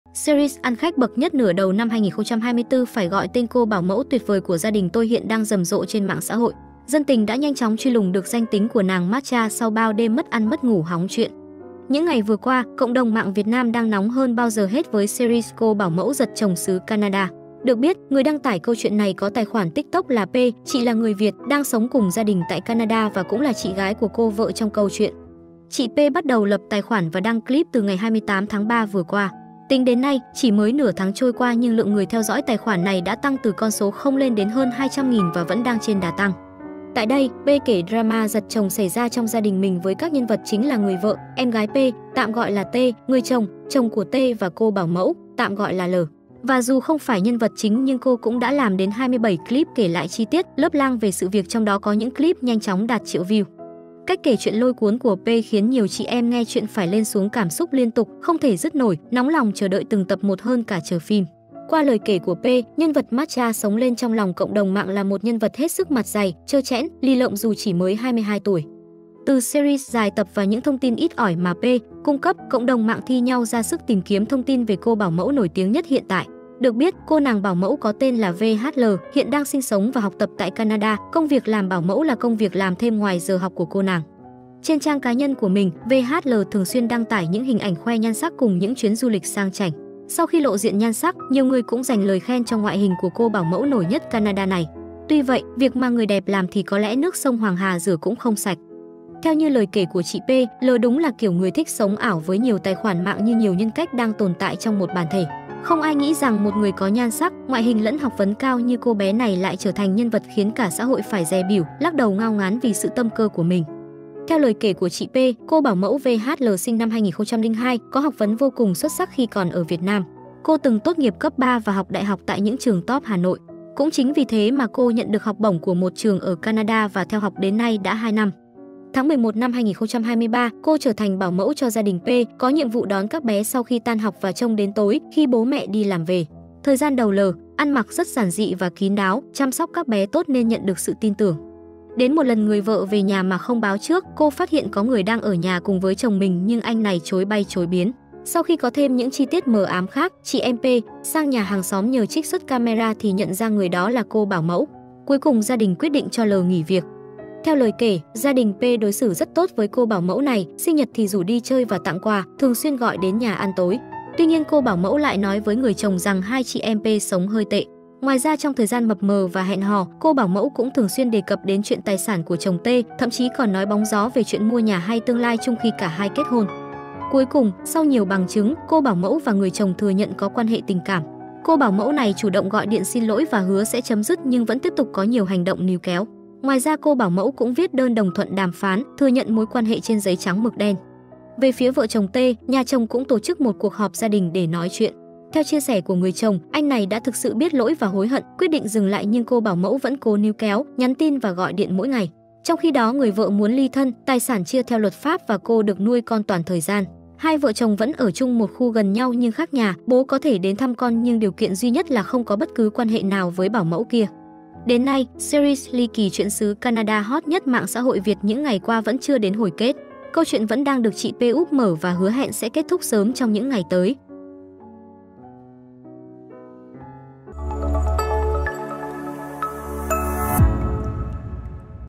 Lộ diện danh tính cô bảo mẫu hot nhất Canada
Series ăn khách bậc nhất nửa đầu năm 2024 phải gọi tên "cô bảo mẫu tuyệt vời của gia đình tôi" hiện đang rầm rộ trên MXH. Dân tình đã nhanh chóng truy lùng được danh tính của nàng “matcha” sau bao đêm mất ăn mất ngủ hóng chuyện.

Những ngày vừa qua, cộng đồng mạng Việt Nam đang nóng hơn bao giờ hết với series cô bảo mẫu giật chồng xứ Canada. Được biết, người đăng tải câu chuyện này có tài khoản TikTok là P., chị là người Việt, đang sống cùng gia đình tại Canada và cũng là chị gái của cô vợ trong câu chuyện.